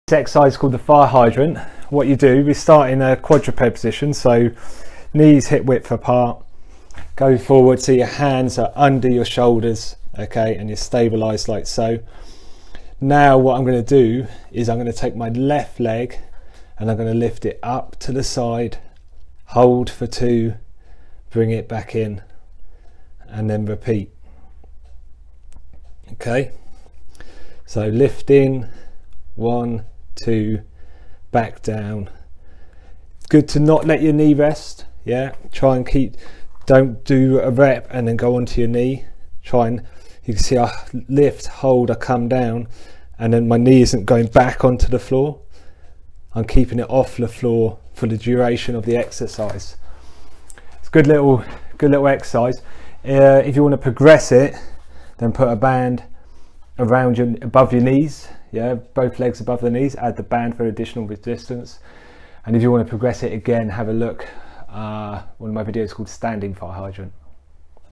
Fire hydrant exercise
Fire hydrant exercise is part of a stage 1 protocol of Patellofemoral Pain Syndrome. (PFPS) It is also a fantastic hip exercise that can also be used in the rehab of piriformis syndrome.

The exercise is performed on all 4's. You draw in or brace your abdominals, Lift your leg out to the side and hold for 2 seconds. This exercise is great for core stability and hip strength.

PFPS is a common condition that particularly can effect runners. It has an insidious onset (gradual over time with no real incident to cause it) and can become debilitating. It normally shows itself with knee pain when squatting, knee pain after running, knee pain whilst running, knee pain when bending and knee pain after squats.

PFPS knee pain is treatable and I have put together a range of PFPS exercises. these will all be placed in a playlist or hosted on my